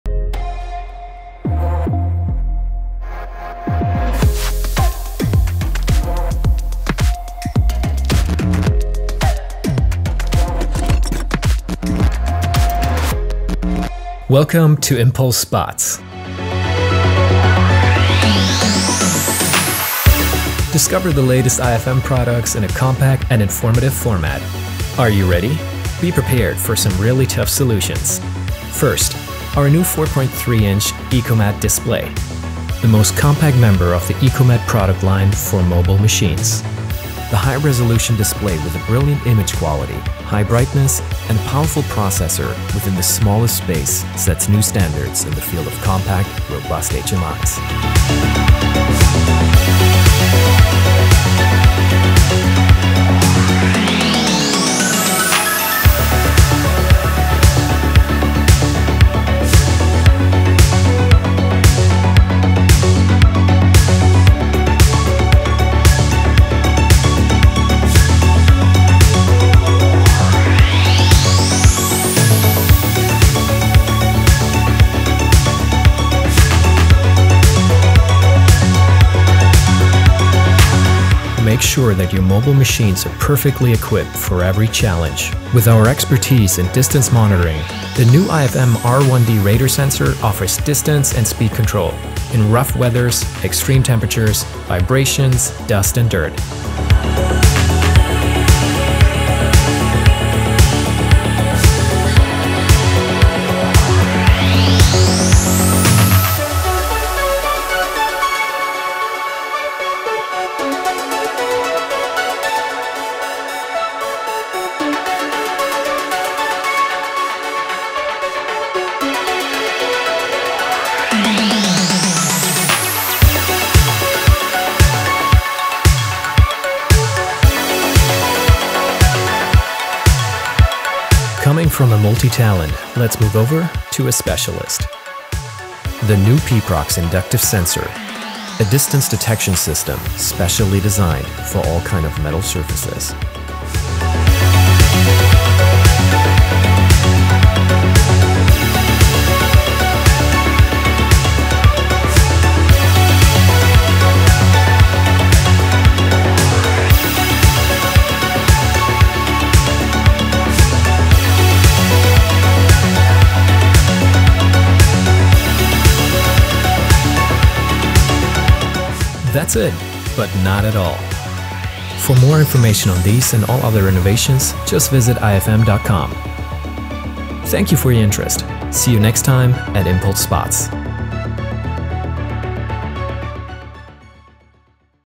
💡 Discover the latest innovations from ifm | Impulse Spots #2
In today's episode of the Impulse Spots we present the latest innovations from ifm. You will get an overview and all the necessary information about the latest releases. These include the 4.3” ecomatDiplay with a high processing power and memory space, R1D radar sensor for distance monitoring even at bad sight and the P|Prox inductive sensor which is adaptable to custom targets via fast 1-point or 3-point calibration.